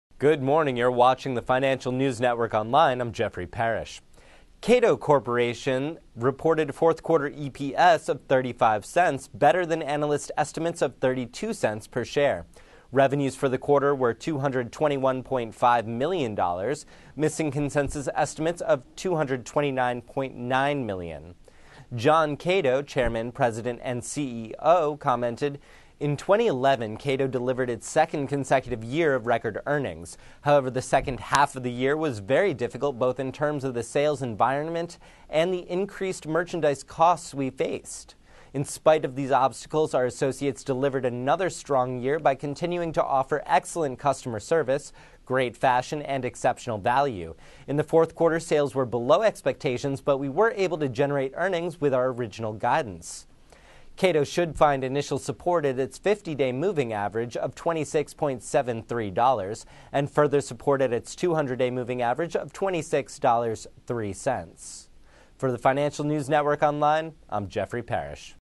Cato Corporation Reports Mixed Earnings in Q4
Cato Corporation (NYSE:CATO) reported Q4 EPS of $0.35, better than analyst estimates of $0.32 per share. Revenues for the quarter were $221.50 million, missing consensus estimates of $229.90 million.

John Cato, Chairman, President and Chief Executive Officer commented, "In 2011, Cato delivered its second consecutive year of record earnings.  However, the second half of the year was very difficult both in terms of the sales environment and the increased merchandise costs we faced. In spite of these obstacles, our associates delivered another strong year by continuing to offer excellent customer service, great fashion and exceptional value. In the fourth quarter, sales were below expectations but we were able to generate earnings within our original guidance."

Cato should find initial support at its 50-day moving average (MA) of $26.73 and further support at its 200-day MA of $26.03.